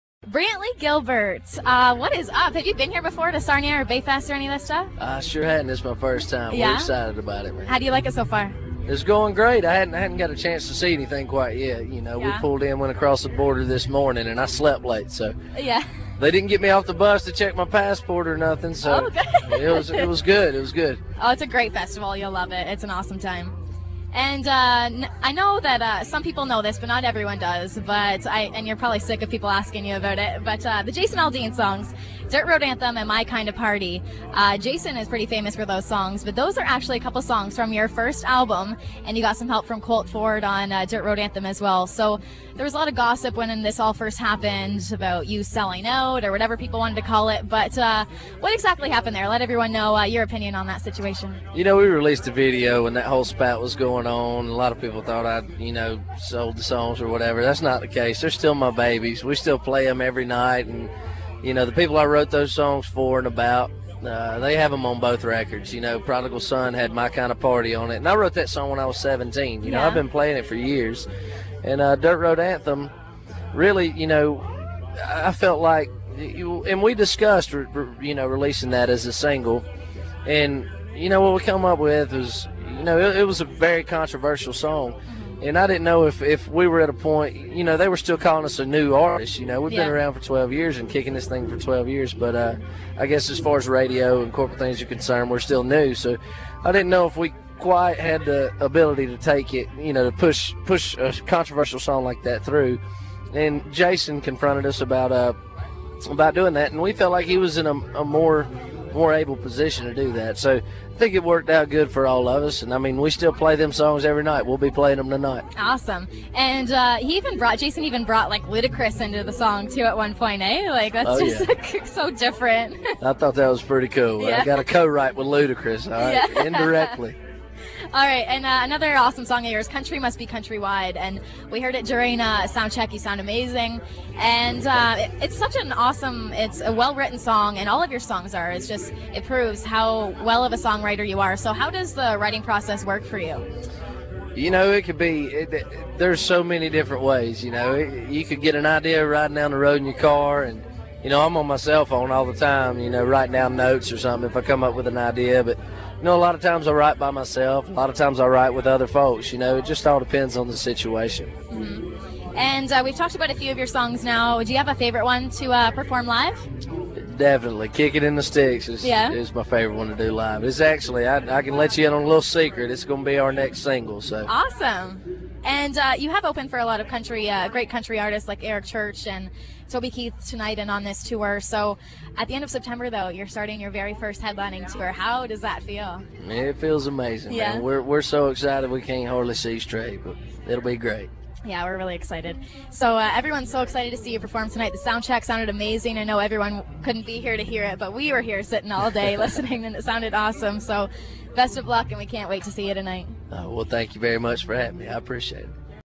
Brantley Gilbert interview at Sarnia Bayfest
Amberli got to interview Brantley Gilbert backstage at Sarnia Bayfest 2012!